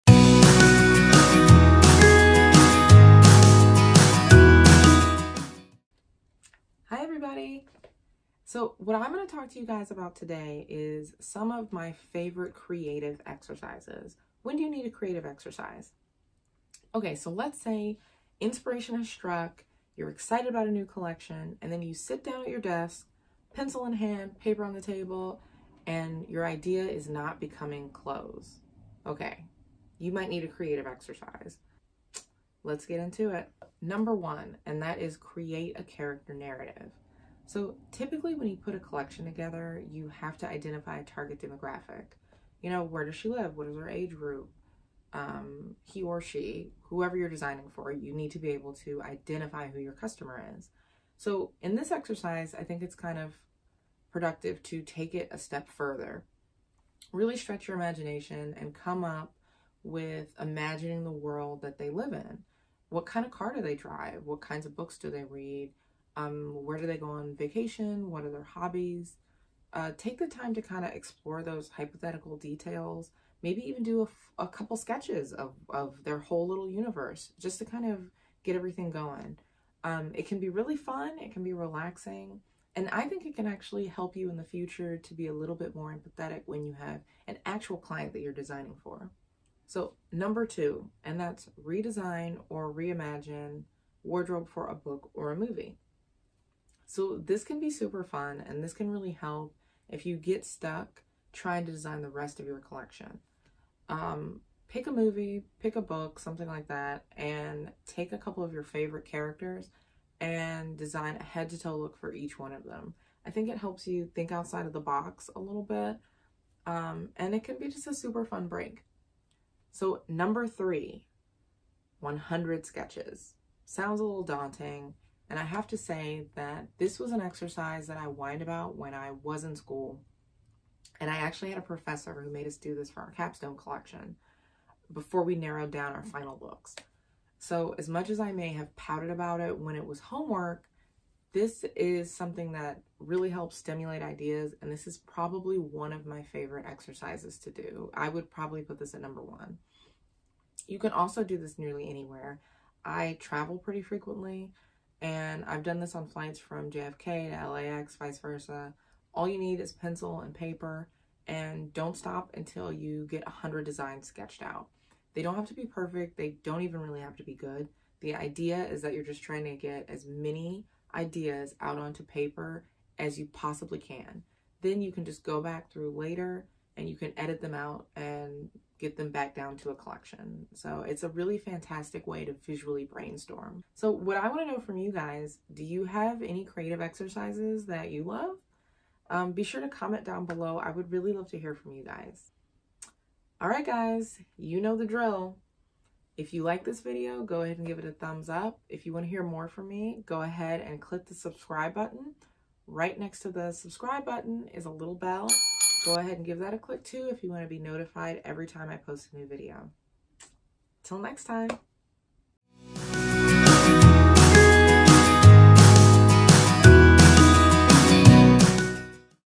3 of My Favorite Creative Exercises!
Today, I talk about 3 of my favorite creative exercises to do whenever I’m feeling stuck designing a collection. These are great to do if you just want to practice or warm up before getting started, as well as if you’re halfway through and are feeling stumped!

What are some of your favorite ways to exercise your creativity?